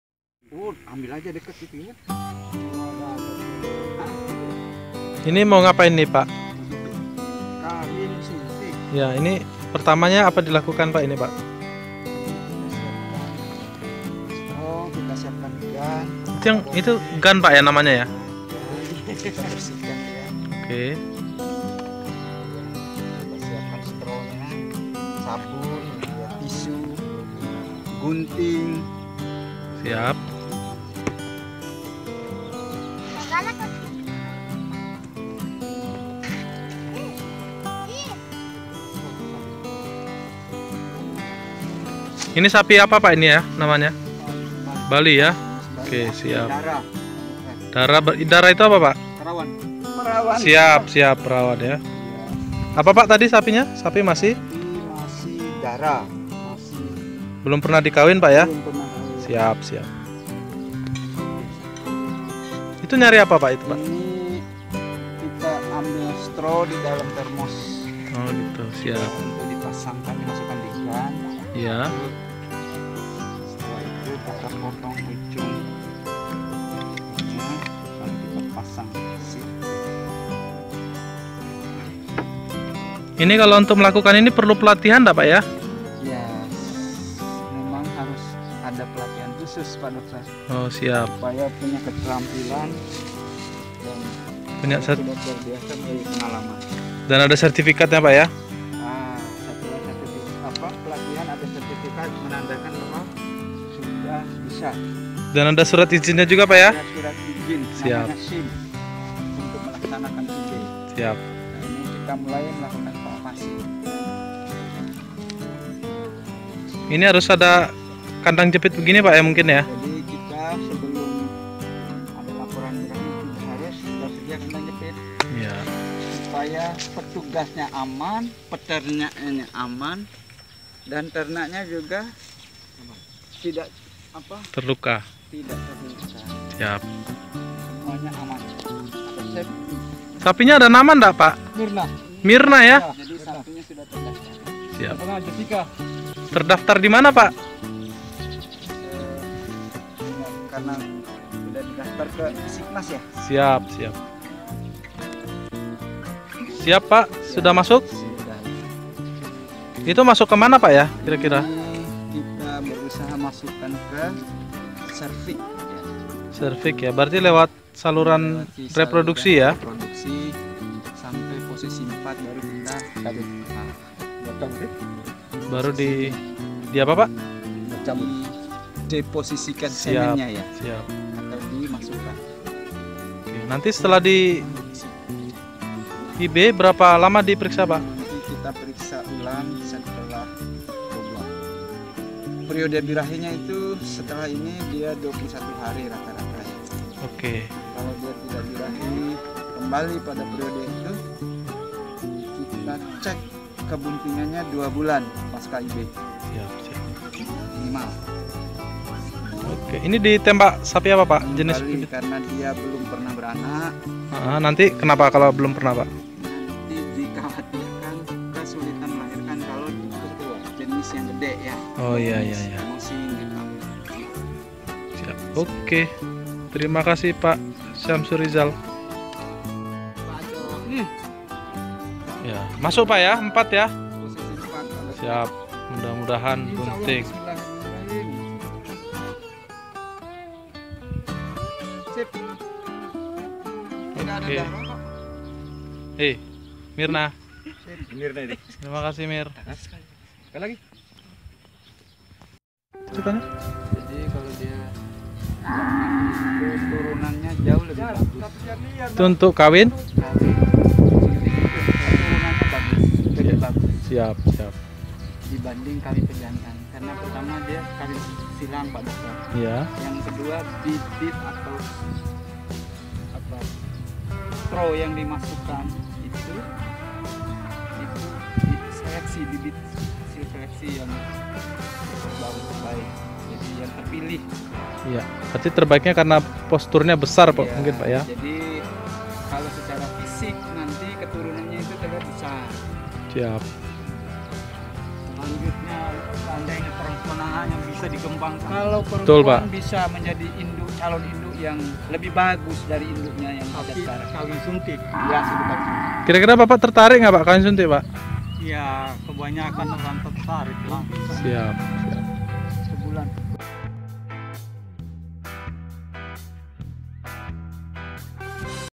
Kawin suntik (Inseminasi Buatan) pada sapi di Tarakan
Vlog kegiatan lapangan di Tarakan Kalimantan Utara dalam rangkap UPSUS SIWAB 2018.